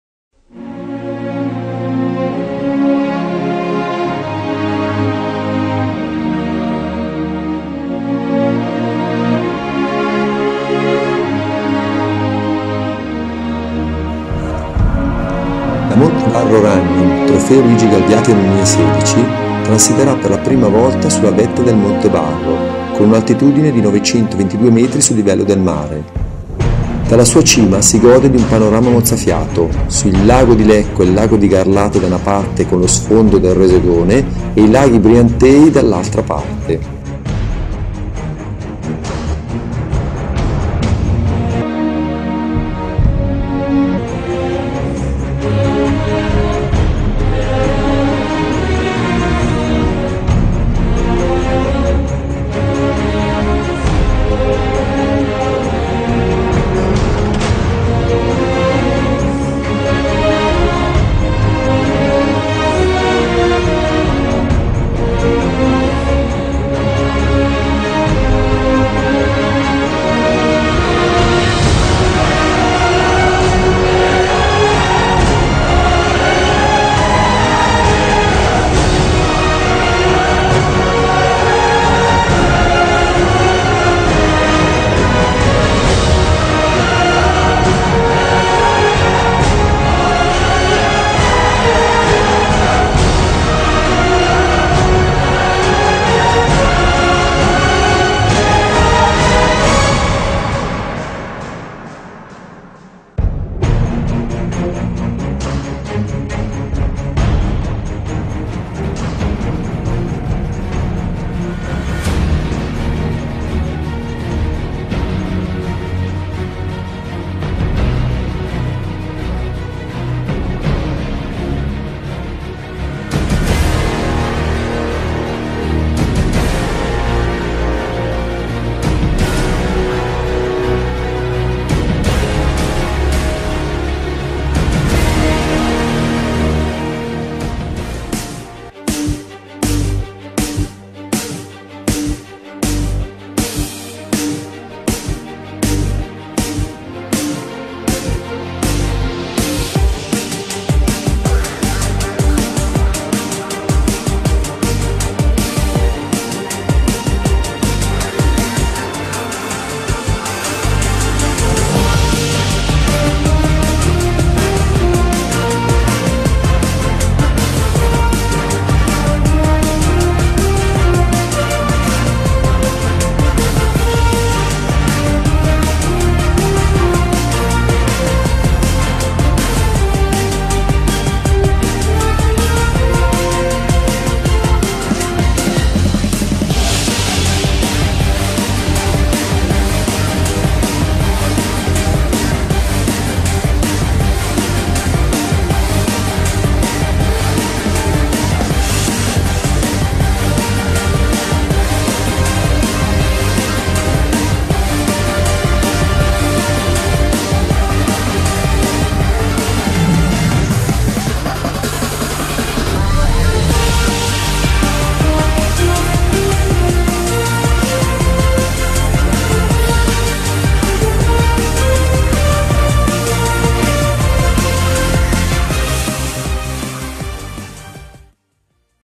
MONTE BARRO RUNNING - TROFEO LUIGI GALBIATI 2016 PASSAGGIO IN VETTA
Video promozionale della modifica del tracciato in occasione della 5° edizione della Monte Barro Running-Trofeo Luigi Galbiati che si disputerà Domenica 17 aprile 2016 a Galbiate.
L'effetto di schiacciamento dell'immagine della telecamera ripresa dall'alto fa sembrare molto scoscese le pareti della montagna.